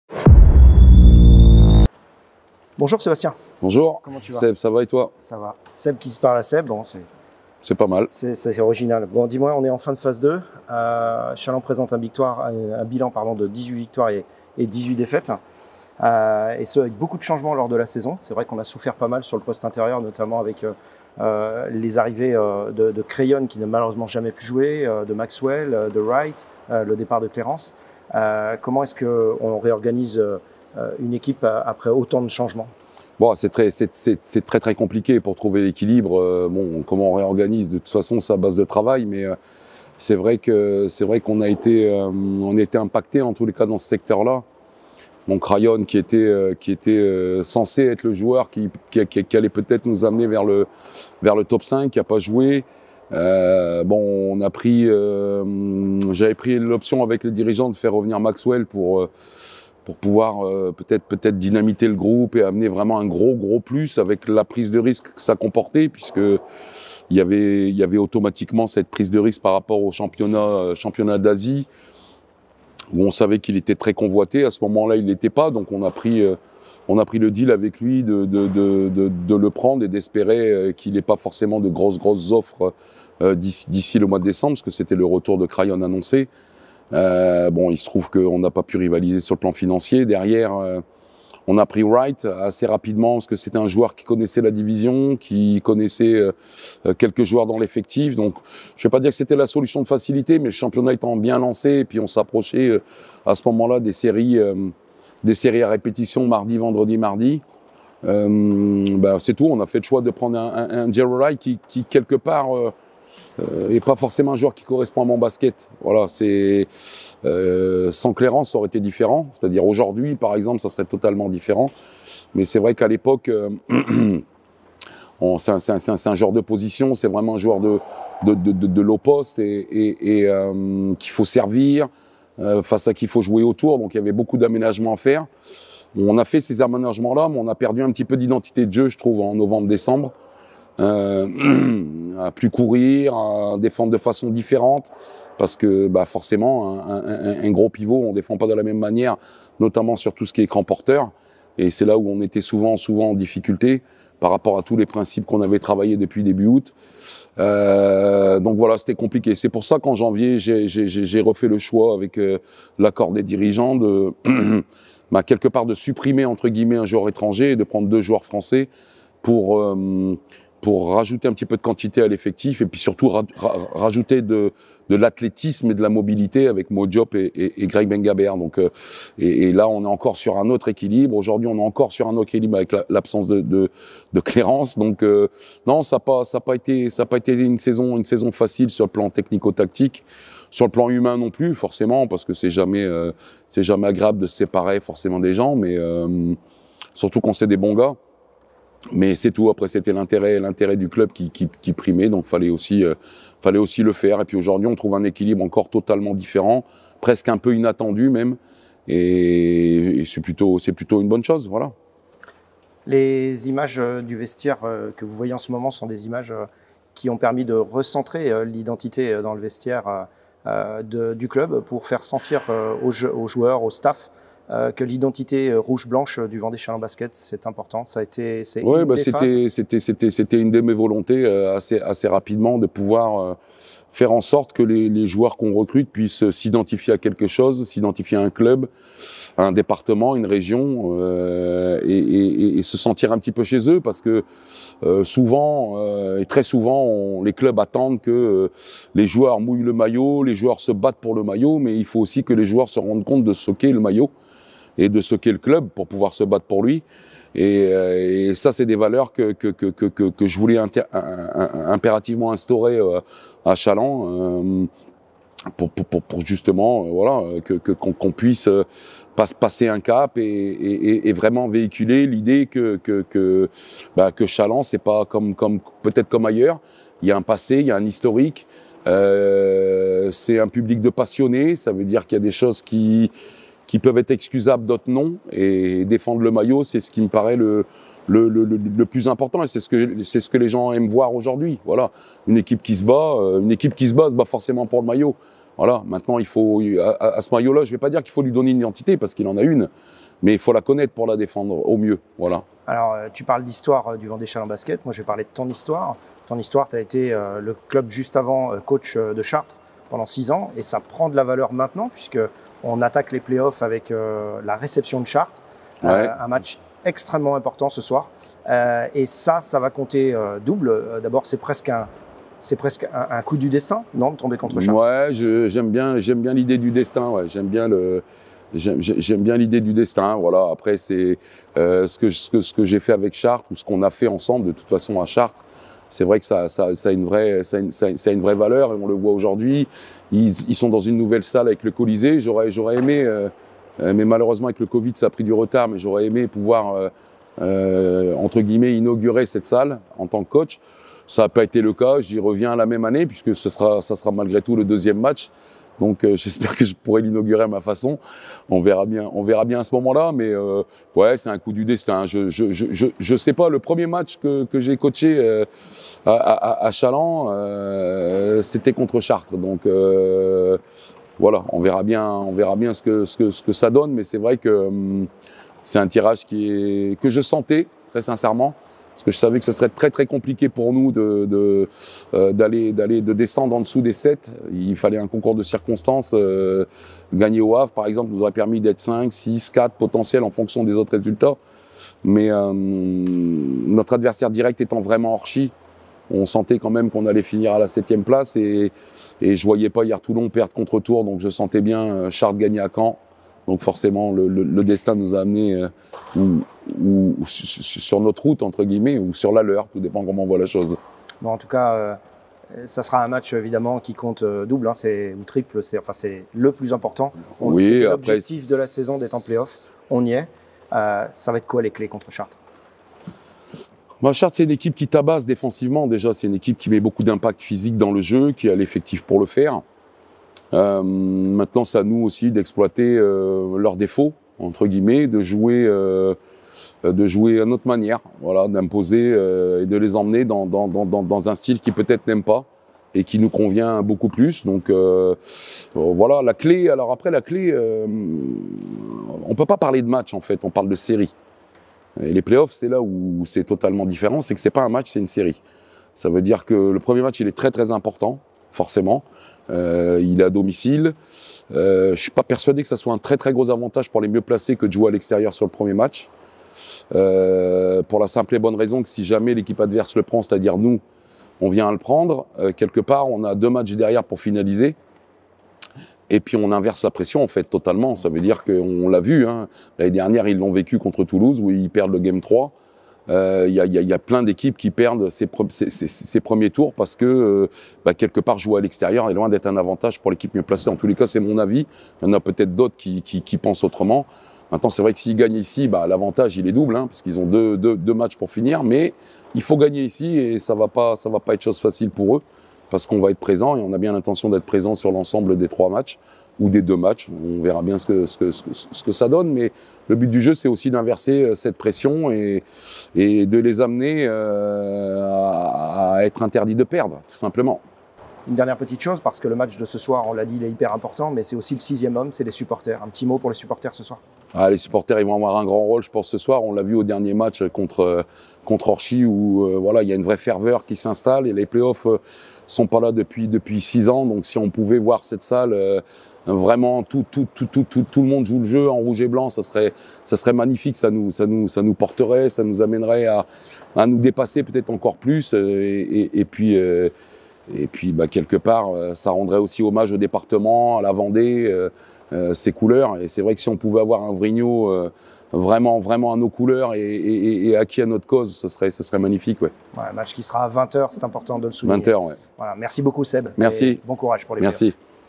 🎤 L'interview d'avant-match VCB / Chartres 🎤
Interview de Sébastien Lambert à quelques heures du coup d'envoi des playoffs.

On vous attends nombreux ce soir!! !🙌💥
TOUS EN ROUGE ET BLANC!!!🔴⚪